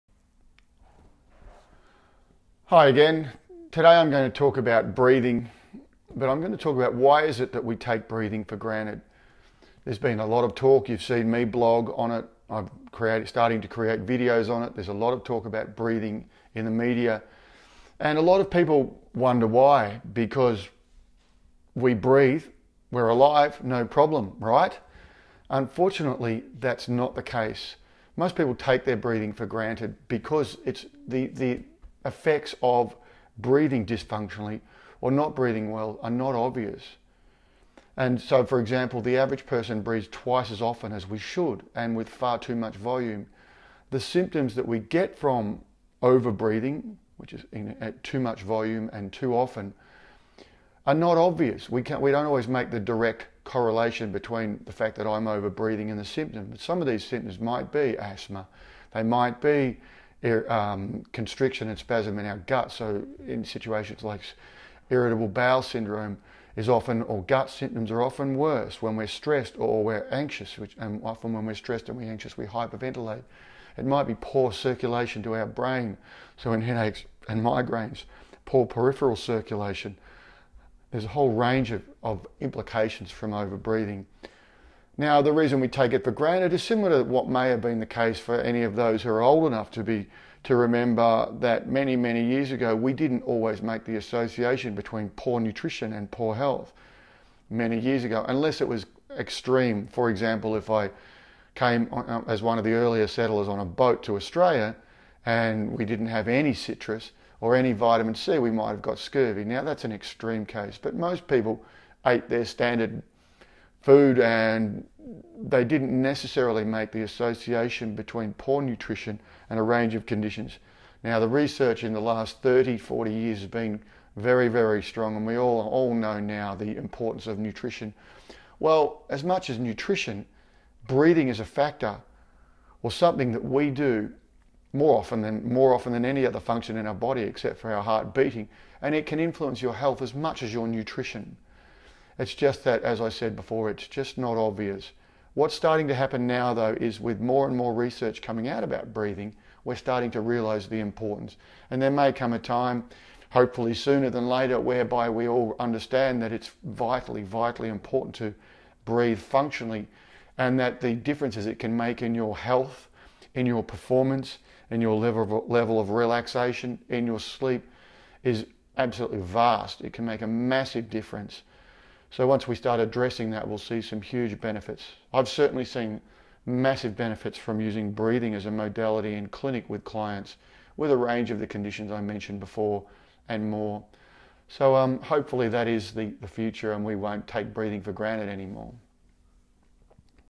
Why do we take our breathing for granted?
A short discussion on breathing and, given it is a bodily function that we perform more than any other, apart from our heart beating, why we don't treat poor breathing as seriously as we would poor nutrition, for example.